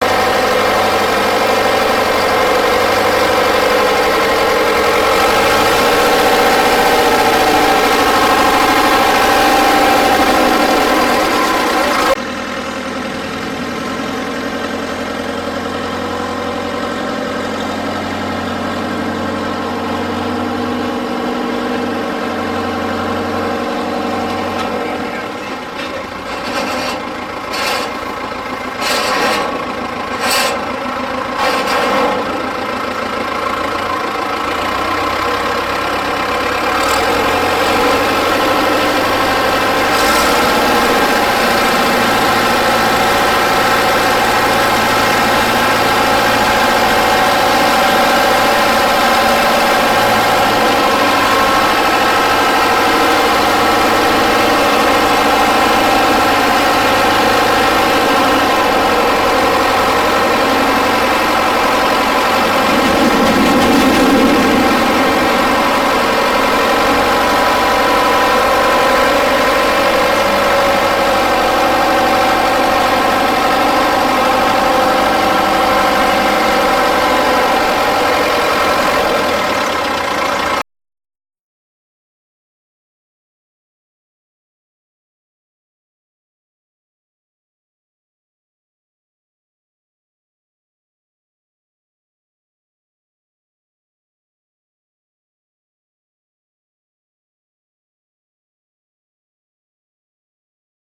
1970 Ford County Forward Control 4WD Tractor Running in Edendale
1970 Ford County Forward Control 4WD Tractor Running at the 2024 Edendale Vintage Machinery Club Crank Up in Edendale, in the Southland Region of New Zealand's South Island.